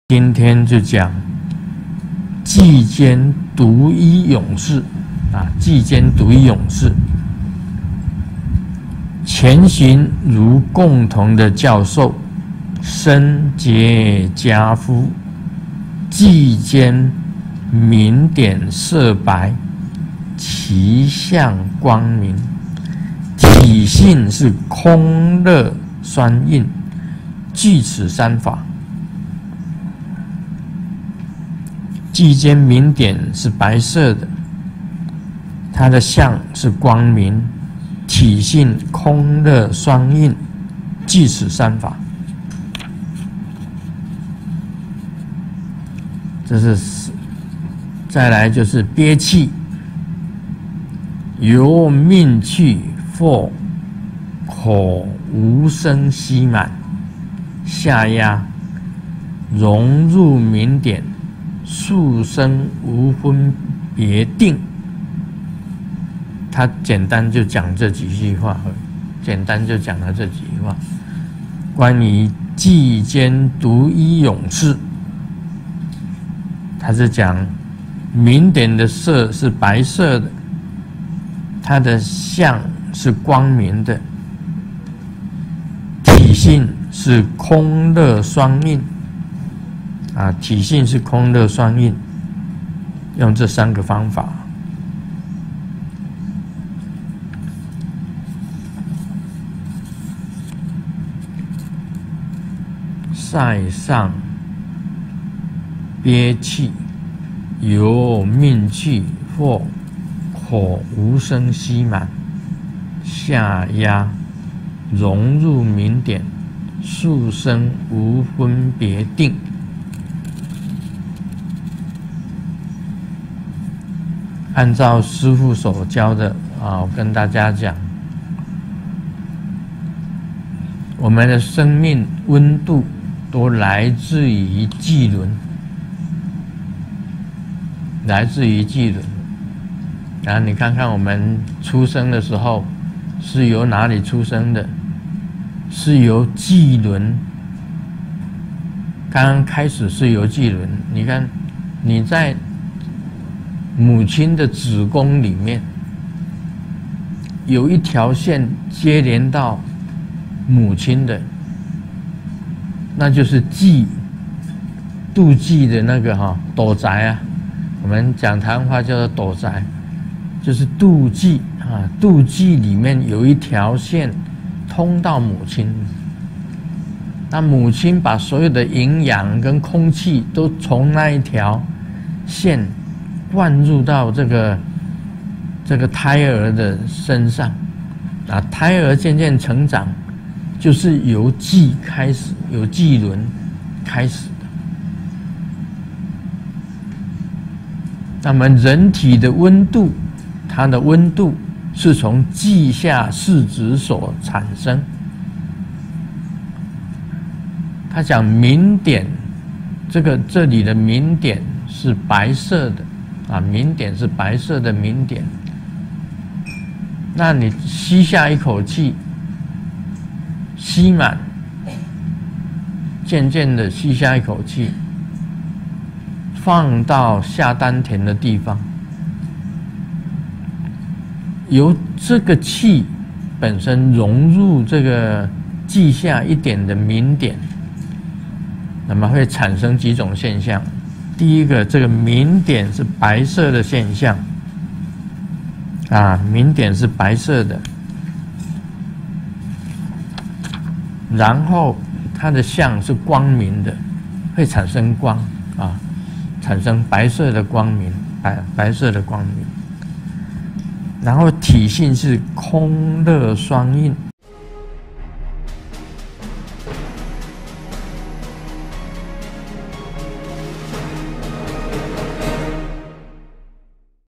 20180805聖尊蓮生活佛盧勝彥開示「道果-密灌 -五不依輪-明點瑜伽-臍間獨一勇士」：明點是白色及光明的
20180805聖尊蓮生活佛盧勝彥開示「道果-密灌 -五不依輪-明點瑜伽-臍間獨一勇士」：明點是白色及光明的